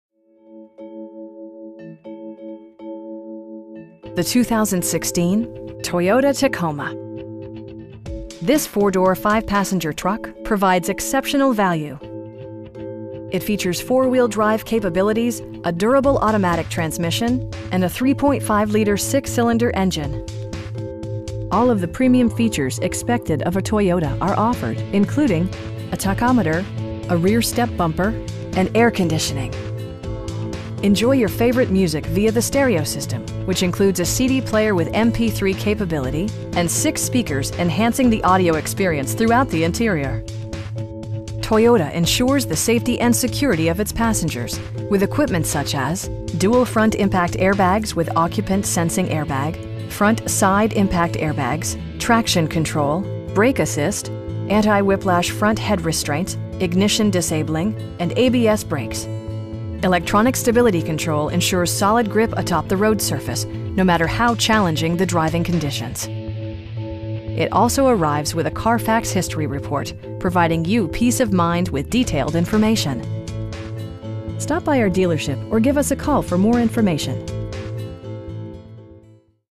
2016 Toyota Tacoma TRD Offroad in Muskegon, MI 49444
The My Auto Group

The 2016 Toyota Tacoma. This 4 door, 5 passenger truck provides exceptional value! It features four-wheel drive capabilities, a durable automatic transmission, and a 3.5 liter 6 cylinder engine. All of the premium features expected of a Toyota are offered, including: 1-touch window functionality, a tachometer, a rear step bumper, tilt and telescoping steering wheel, power windows, an overhead console, and air conditioning. Toyota ensures the safety and security of its passengers with equipment such as: dual front impact airbags with occupant sensing airbag, front side impact airbags, traction control, brake assist, anti-whiplash front head restraints, ignition disabling, and ABS brakes. Electronic stability control ensures solid grip atop the road surface, no matter how challenging the driving conditions. It also arrives with a Carfax history report, providing you peace of mind with detailed information. We know that you have high expectations, and we enjoy the challenge of meeting and exceeding them! Stop by our dealership or give us a call for more information.